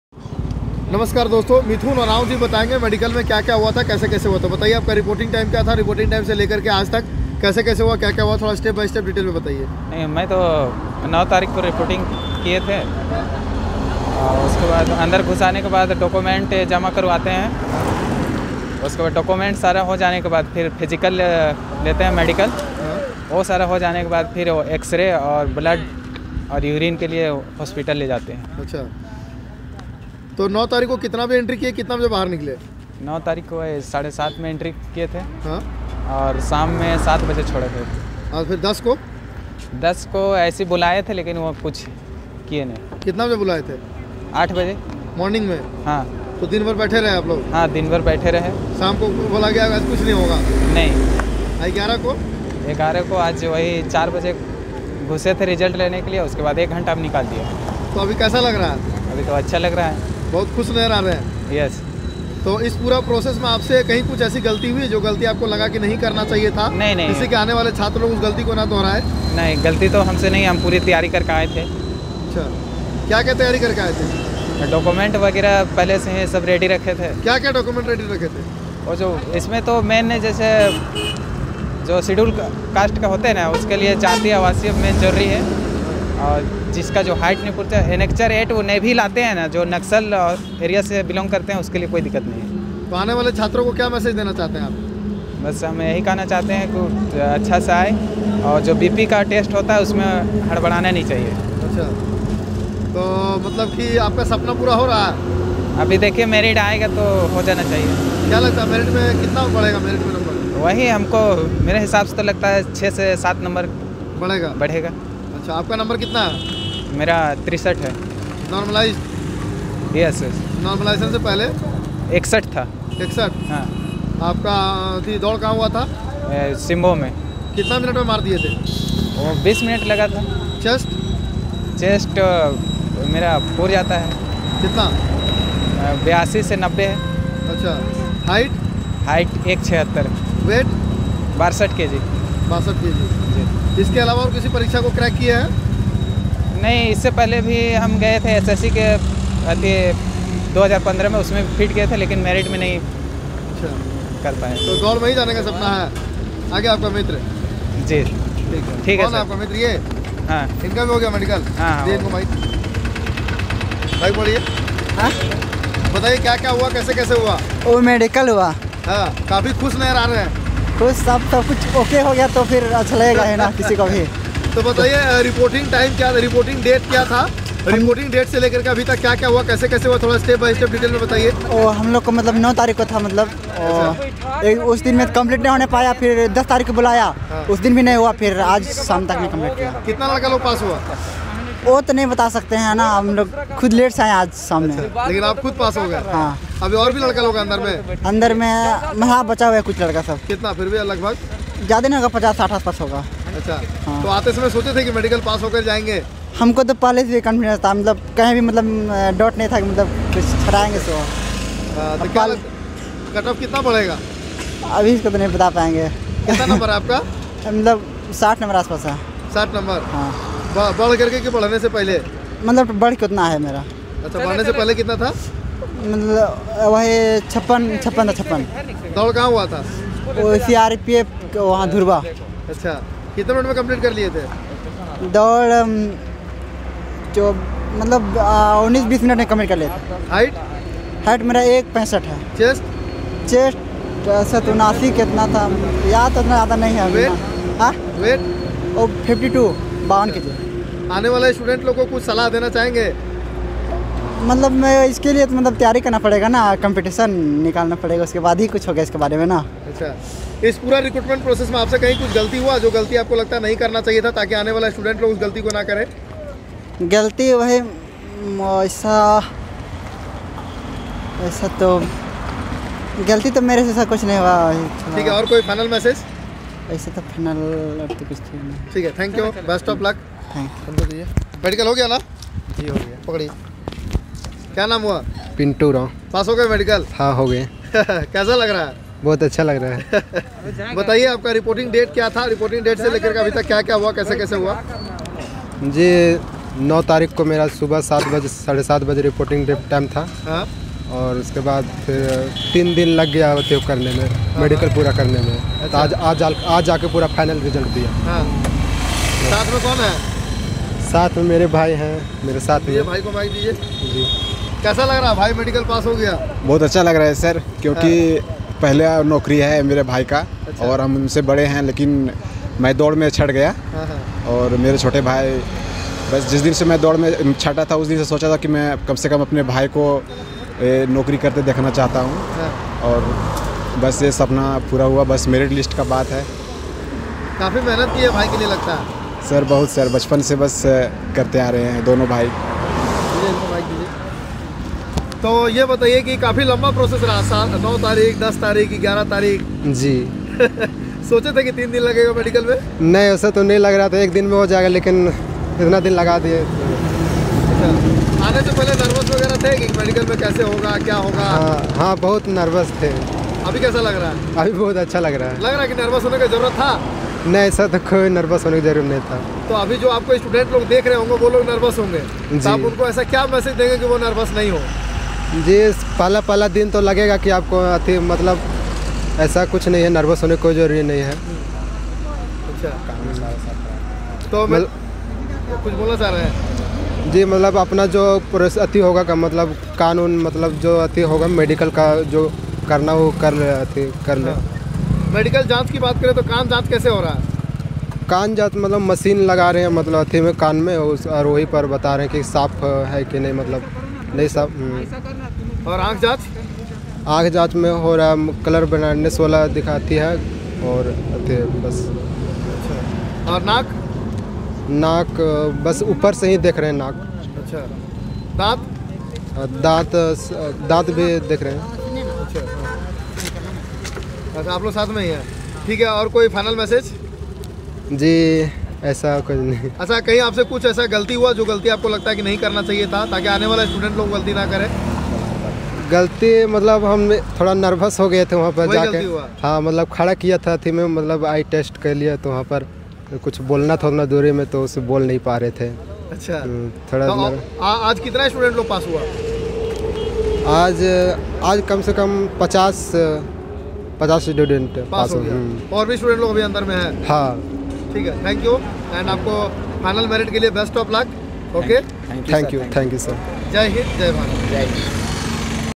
SSC GD Medical Latest Video || Nervousness को कैसे दूर करें? || PASS Candidate Interview
सभी मॉक टेस्ट पर 15% तत्काल छूट पाने के लिए कूपन कोड 'UMANGSTUDY' लागू करें।